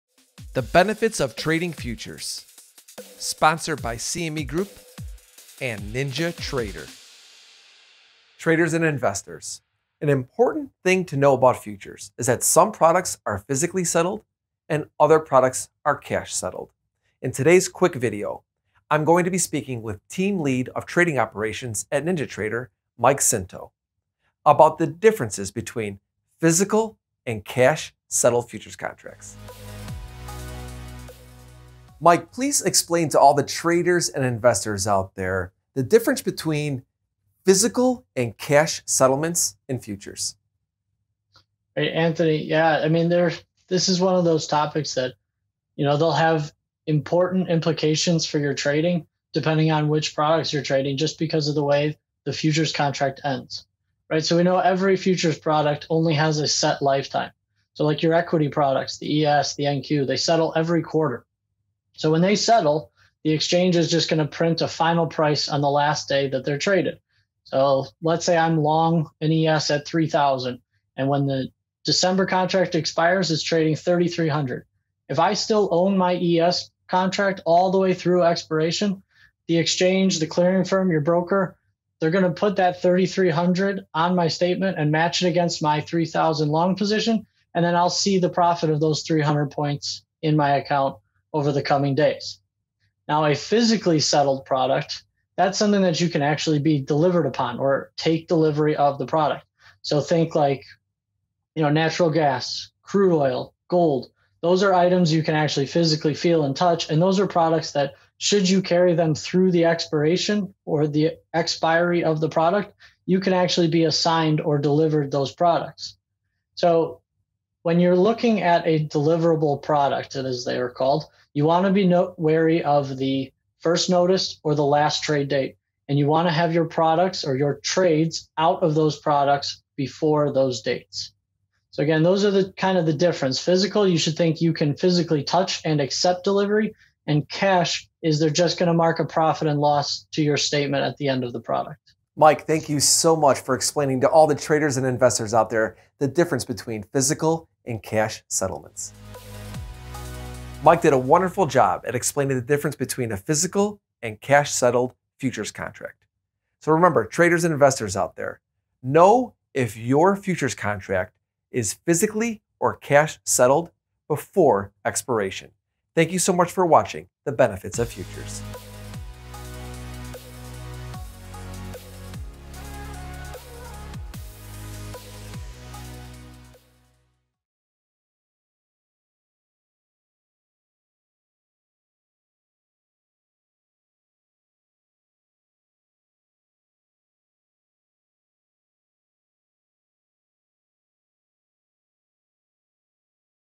Explaining Cash and Physical Settlement in Futures
What’s the difference between cash settled and physically delivered futures? Anthony Crudele explains cash and physical settlement as it relates to futures trading in this video!